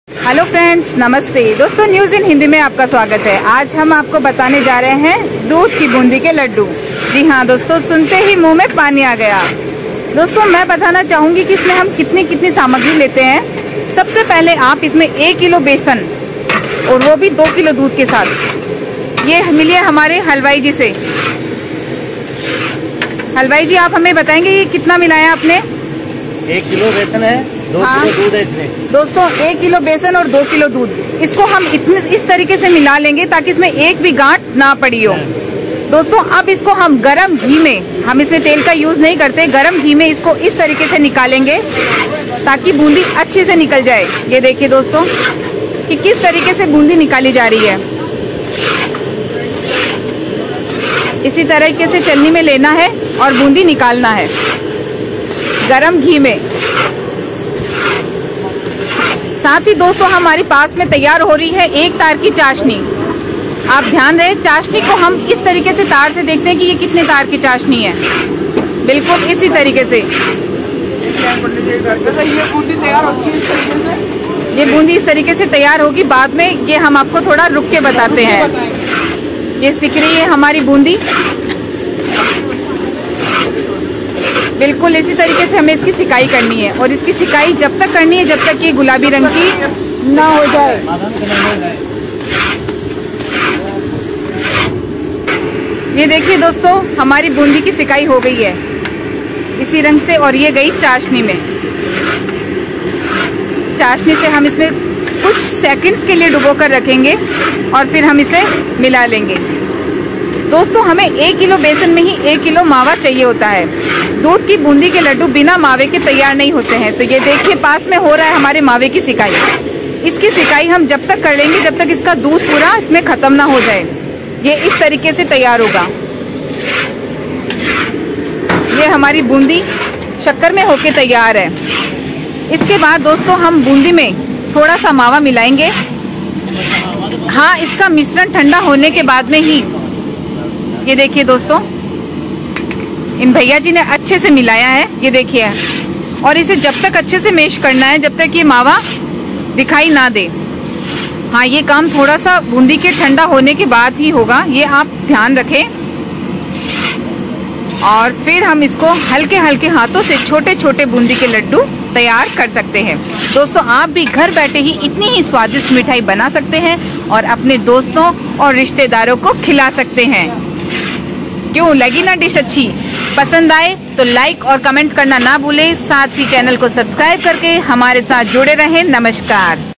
देसी घी में बने दूध के लडडू //how to make dudh ke laddo
दोस्तों आज हम आपको बताने जा रहे हैं देसी घी के बने दूध के लडडू तो इसमें आप दूध से बने लड्डू के बारे में देखेंगे कि उन्हें किस्त इसमें हम 1 किलो बेसन दूध लेते हैं सकते हैं